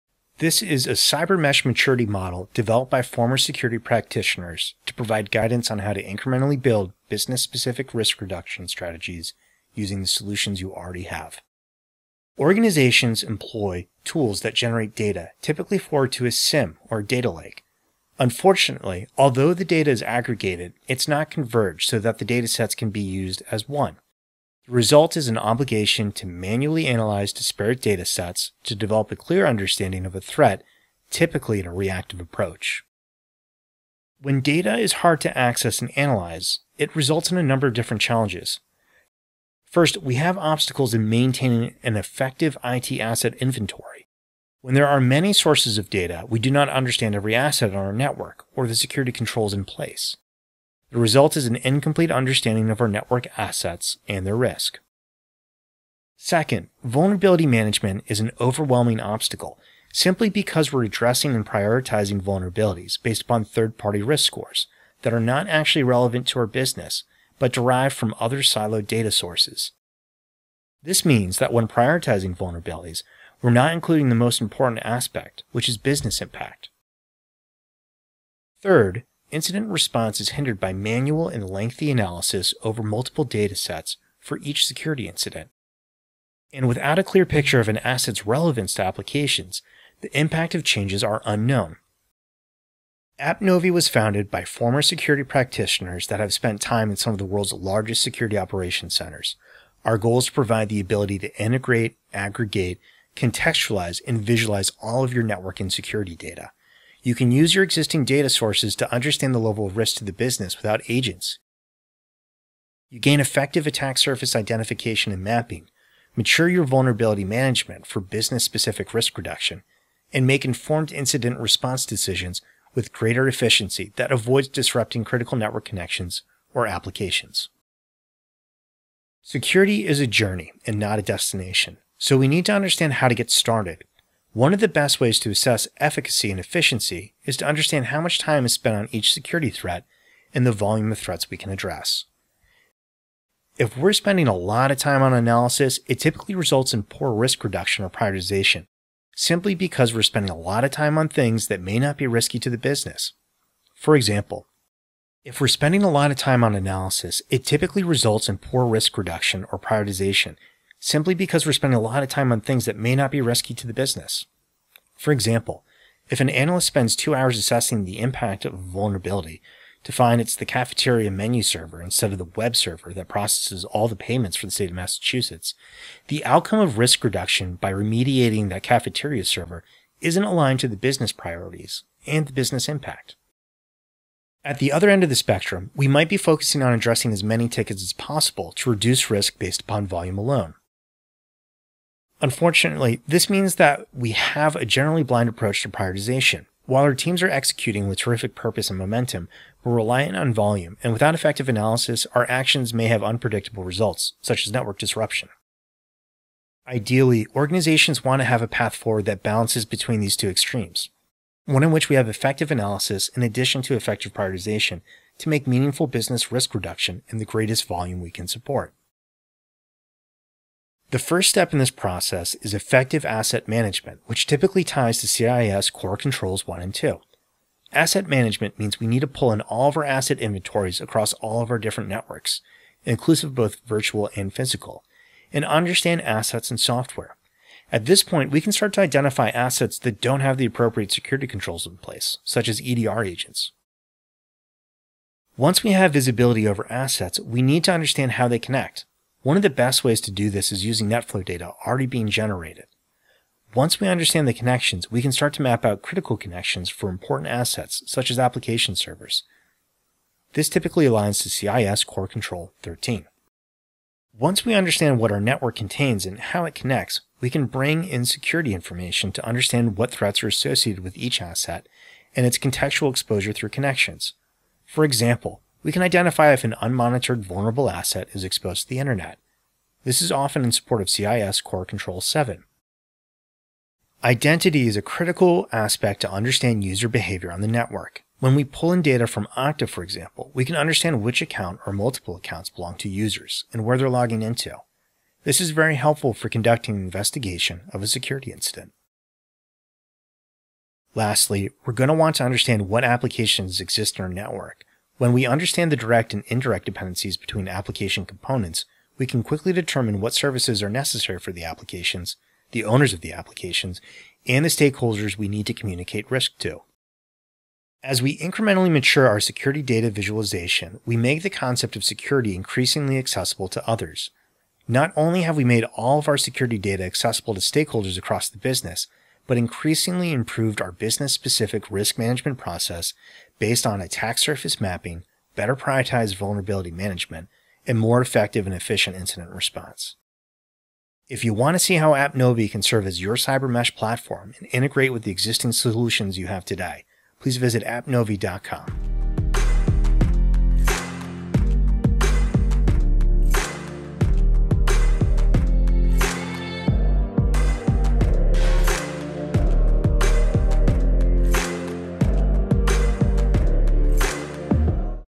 Cybersecurity Mesh Maturity Model
As network complexity increases so does tooling to manage and secure it. This results in siloed data sources that require manual analysis to understand security posture and identify gaps in controls.

To reduce complexity and increase efficiencies, Cybersecurity Mesh Architecture (CSMA) enables different security services to work together. Enterprises use existing data sources to contextually converge siloed data to reduce complexity, provide a consistent security posture, and enable the adoption and implementation of new technologies.

appNovi was founded by former security practitioners to make security accessible. This video highlights an incremental approach and benefits of integration of security tools in a Cybersecurity mesh architecture.